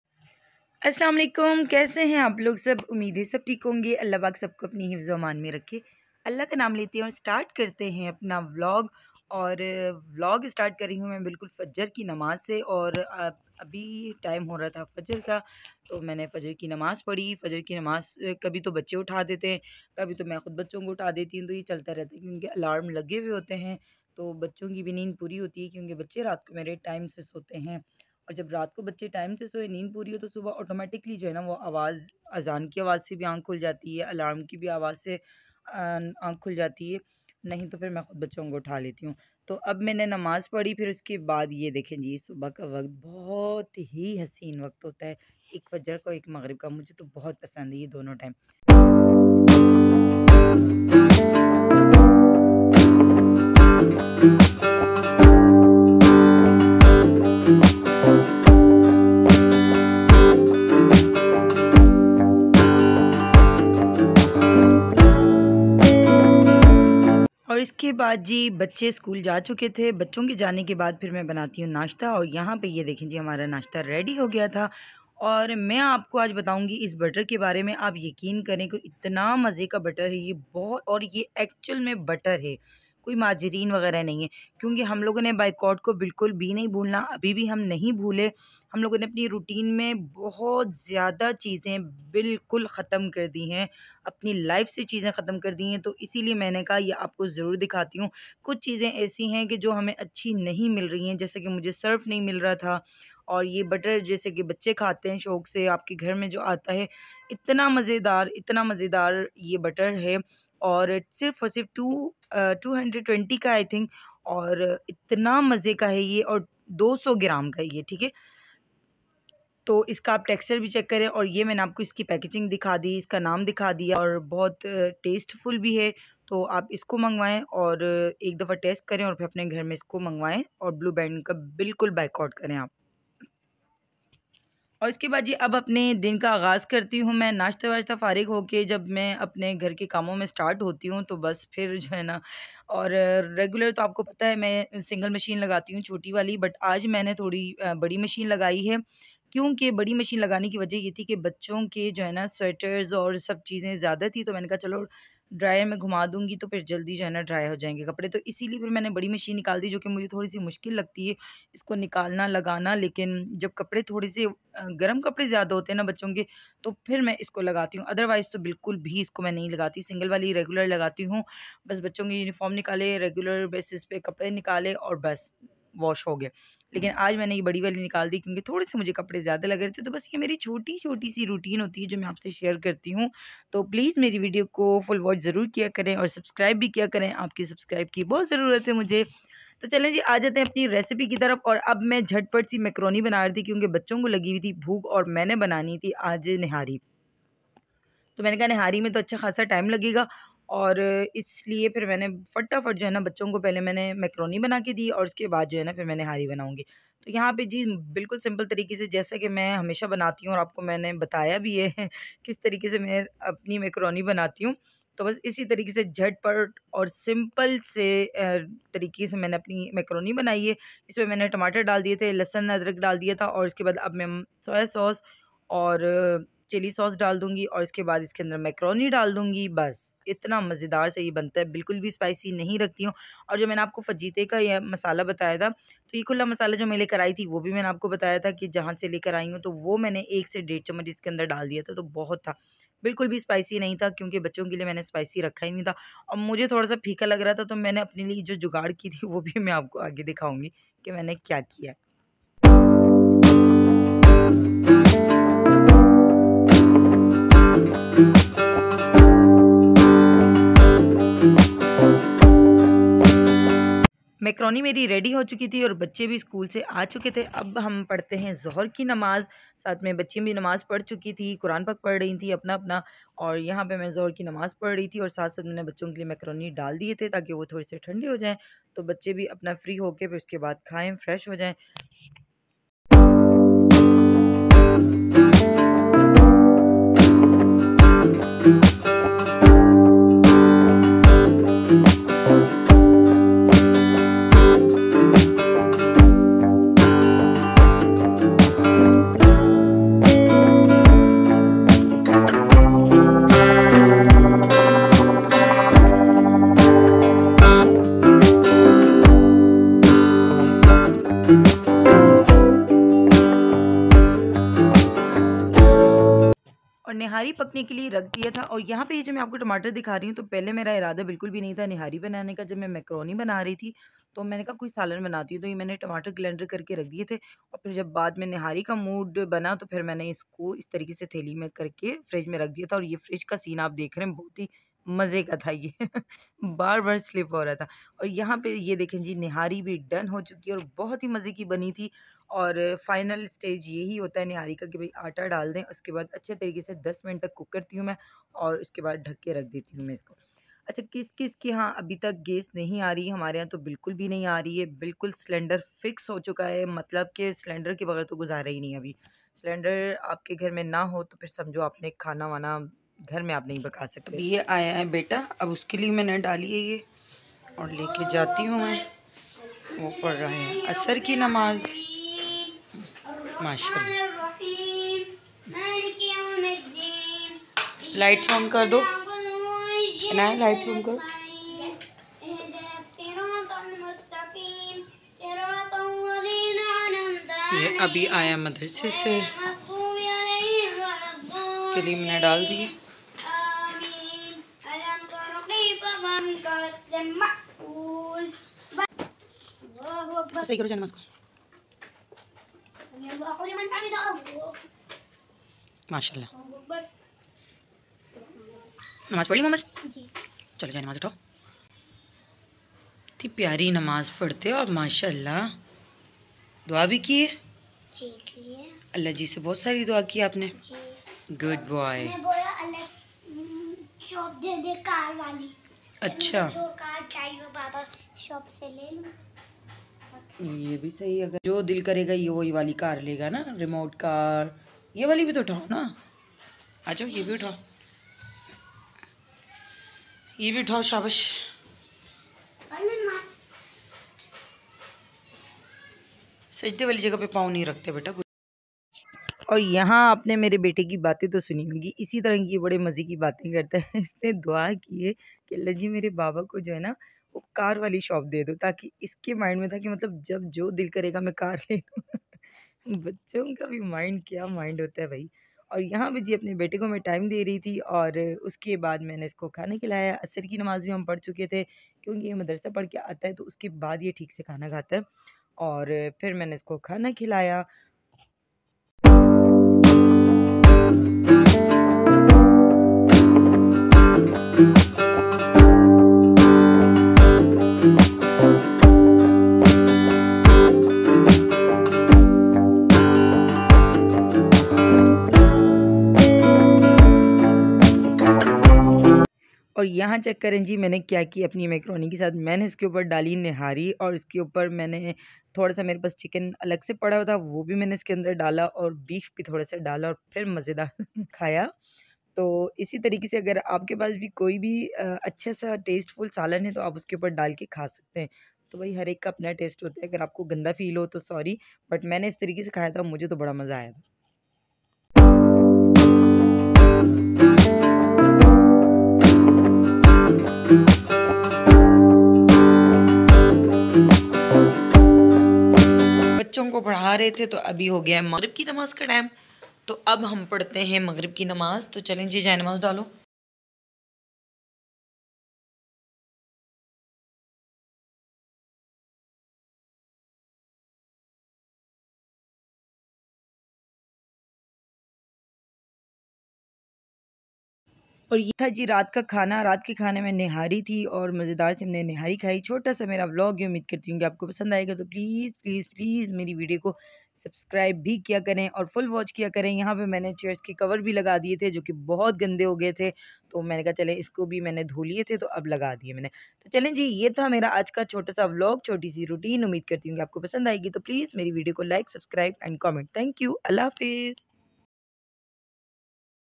Unveiling Macaroni's Delicious Secret | My Full Day Routine @Daily dose
macaroni 😋 and nehare bani aj |  and my full day routine | @Daily dose